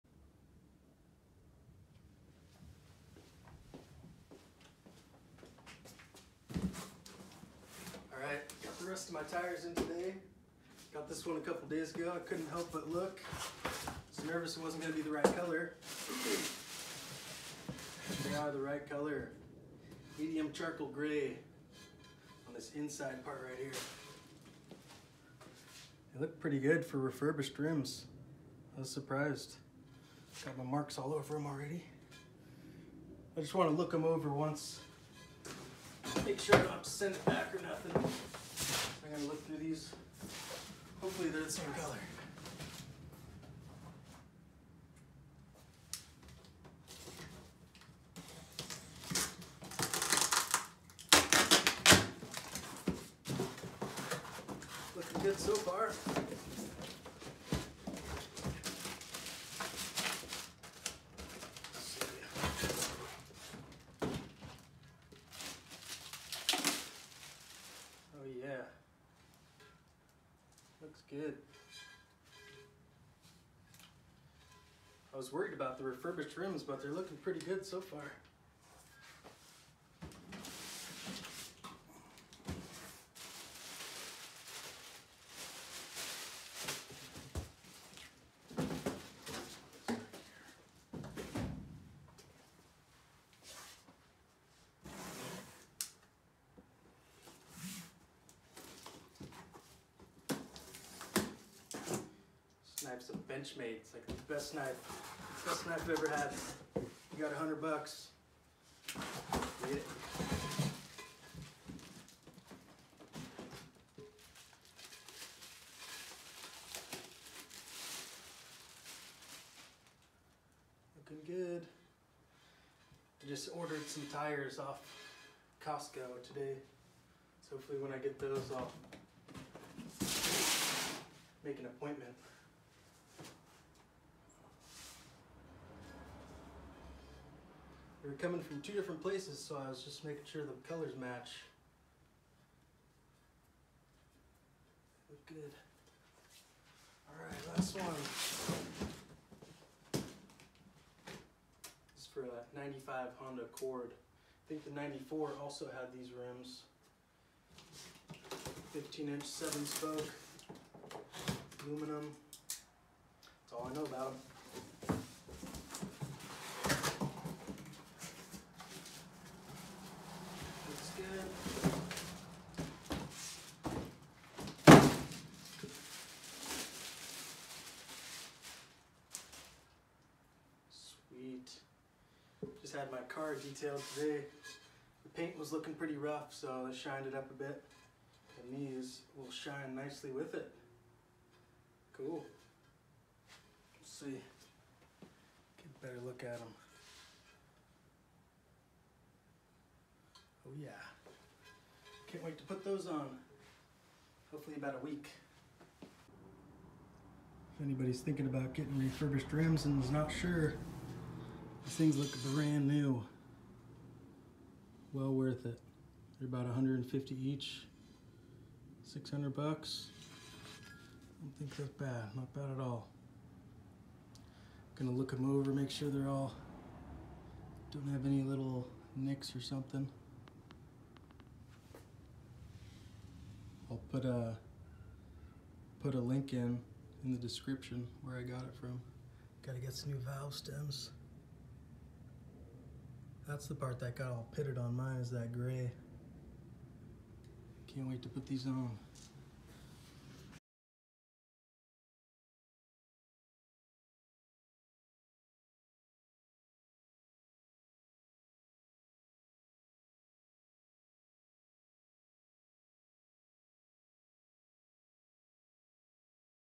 New stock rims for 1995 Honda Accord EX
I ordered a set of refurbished stock rims for my 1995 Honda Accord EX, they're Honda's Medium Charcoal Grey 7 spoke. I've started to clean up the car a bit, give it some life. Check out the other videos below